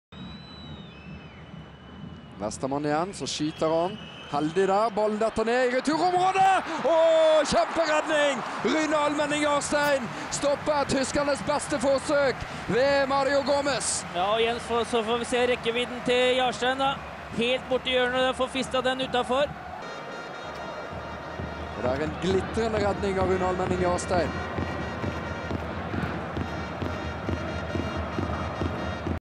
Sjekk Jarsteins syke pantersprang
RBK-keeperen lekte villdyr i Düsseldorf.